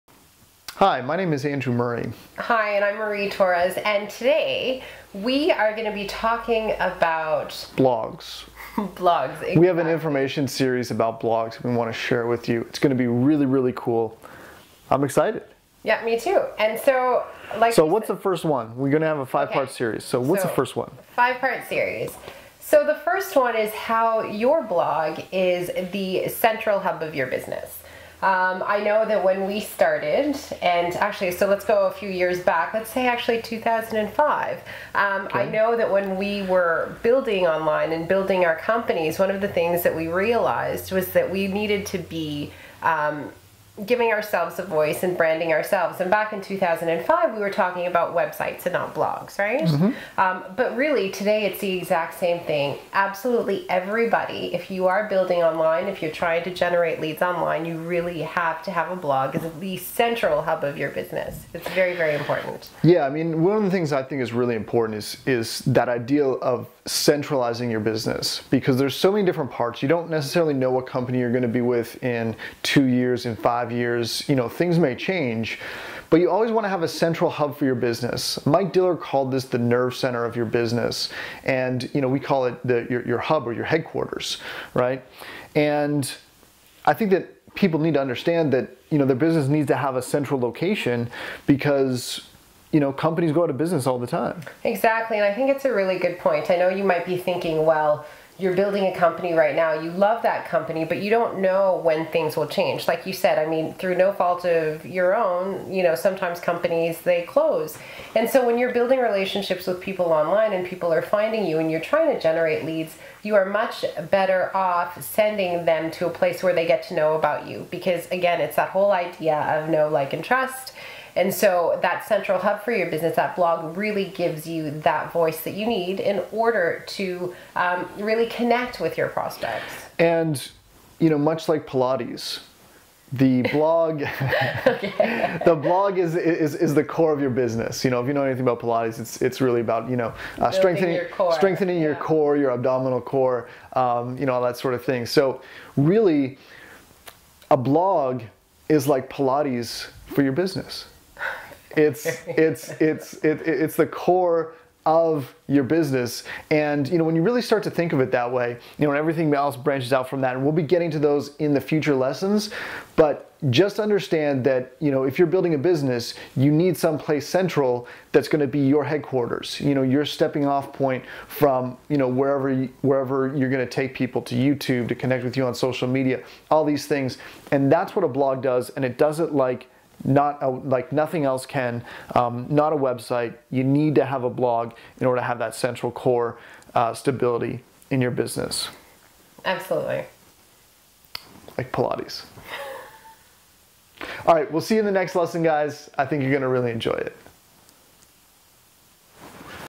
Blog Tip #1 Your Blog Is Your Business HQ
Blog Tip #1 (There are 5 more!):

Why you need a business headquarters and why your blog is the central hub of your online business.

Learn how and why the blog is the central hub of your online business.

I live in 🇨🇦 love playing ⚽️ and cooking 🍳
I invest in crypto and I own a SAAS.

Use Promo: X460EN